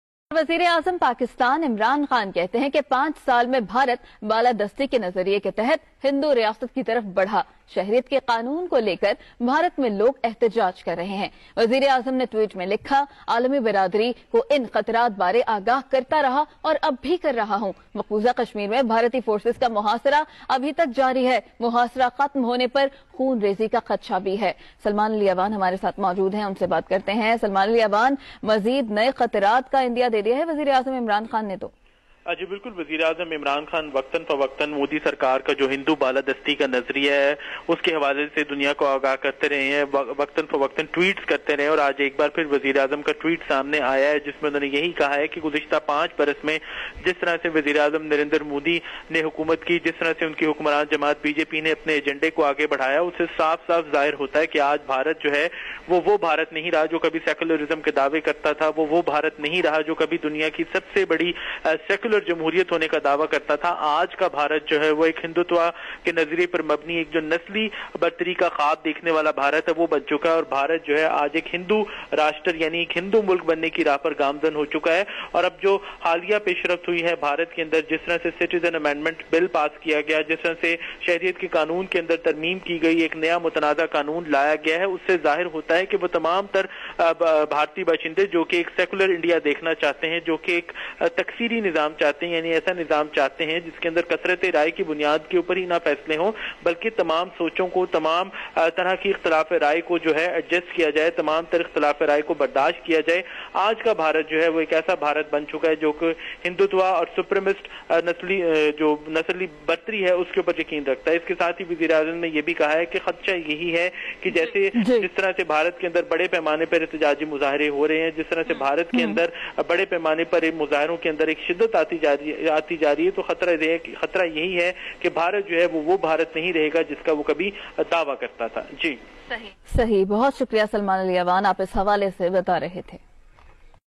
Prime Minister of Pakistan Imran Khan warns international community that Pakistan will befittingly
Prime Minister of Pakistan Imran Khan warns international community that Pakistan will befittingly respond to any Indian false flag operation

“I have been warning the international community of this for some time and am reiterating again: if India does such an operation to divert attention from its domestic chaos plus whip up war hysteria to mobilize Hindu nationalism, Pakistan will have no option but to give a befitting response.
As these protests are increasing, threat to Pakistan from India is also increasing. Indian Army Chief’s statement adds to our concerns of a false flag operation”.

وزیراعظم عمران خان نے خبردار کیا ہے کہ اگر بھارت نے ملک میں جاری فسادات اور ہندوتوا بالادستی کے فروغ کیلئے جنگی جنون بڑھانے کے اقدامات سے توجہ ہٹانے کیلئے کسی قسم کے جعلی فلیگ آپریشن کی کوشش کی تو پاکستان کے پاس اس کا منہ توڑ جواب دینے کے سوا کوئی اور چارہ نہیں ہوگا۔انہوں نے متعدد ٹویٹس میں کہا کہ بھارت میں بڑھتے ہوئے مظاہروں کی وجہ سے پاکستان کو بھارت کی طرف سے درپیش خطرات میں بھی اضافہ ہو رہا ہے۔عمران خان نے کہا کہ بھارتی آرمی چیف کے بیان سے جعلی فلیگ آپریشن کے بارے میں ہماری تشویش میں بھی اضافہ ہورہا ہے۔وزیراعظم نے کہا کہ انہوں نے گزشتہ کچھ عرصے سے عالمی برادری کو اس بارے میں خبردار کیا تھا اور وہ اب اس کا اعادہ کررہے ہیں ۔ انہوں نے کہا کہ مودی حکومت کے گزشتہ پانچ سال کے دور حکومت میں بھارت میں ہندو راشٹرکی طرف آگے بڑھتے ہوئے ہندوتوا بالادستی کے فسطائی نظریے کو فروغ ملا ہے